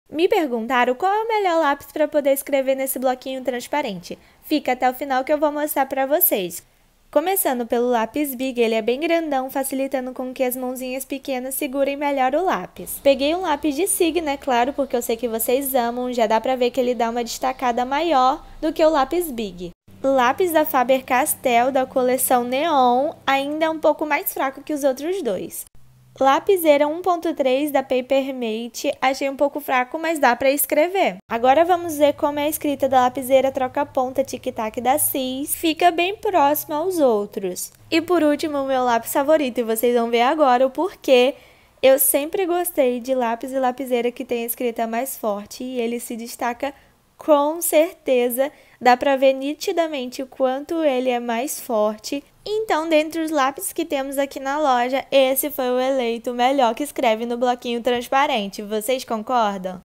Escrevendo no bloquinho transparente 😱
Qual o melhor lápis para escrever no bloquinho transparente?

Testei alguns lápis aqui da loja e elegi o melhor para vocês 😍